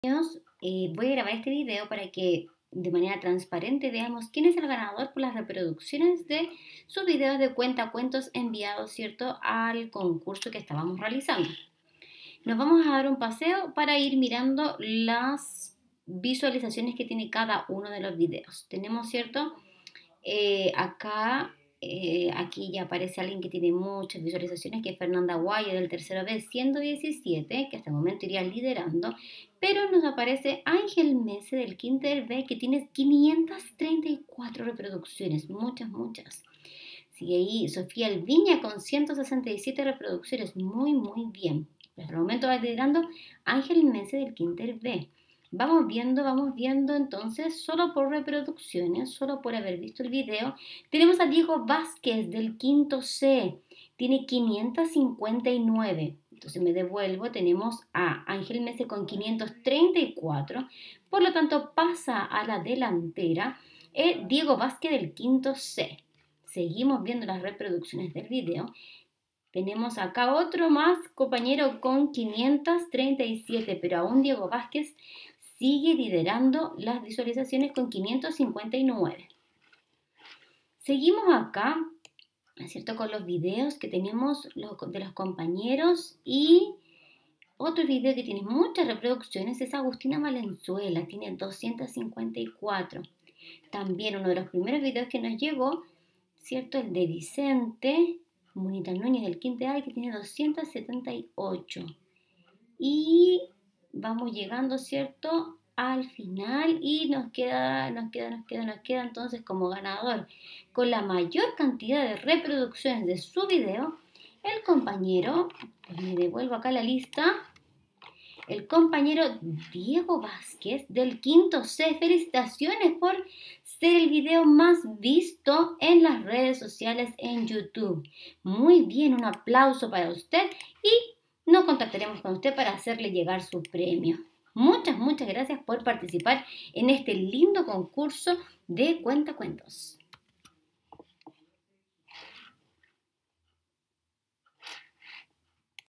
Ganador, reproducciones concurso "Cuéntame tu mejor cuento"
Ya tenemos ganador por la categoría reproducciones del concurso.

Colegio Alcantara de la Cordillera, celebra el mes de libro.

Felicidades por tu popularidad
 Diego Vasquez del 5ºC, tu eres el ganador